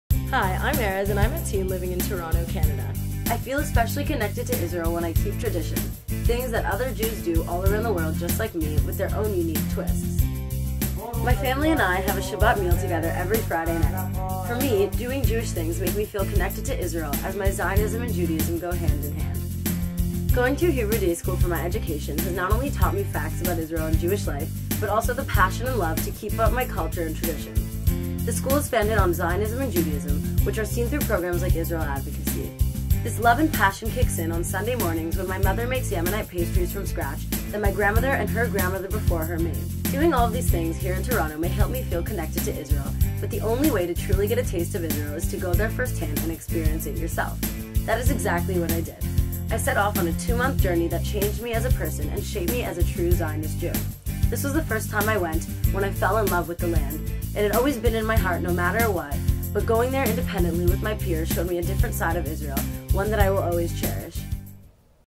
What Does Your Israel Look Like? Israel @65- Erez Zobary
This is my original 65-second video showing my relationship to Israel.
This is for a UJA contest, and I can only win with YOUR help!!!

VOTE FOR ME SO I CAN WIN FREE TICKETS TO ISRAEL!!! :)